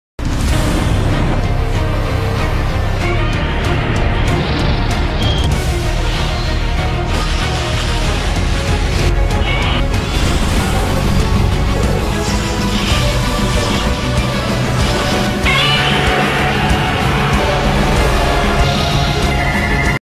Catzilla 2022 (Trailer)
Catzilla Saves us from all evil monsters once again!
Coming Soon To A Theater Near You!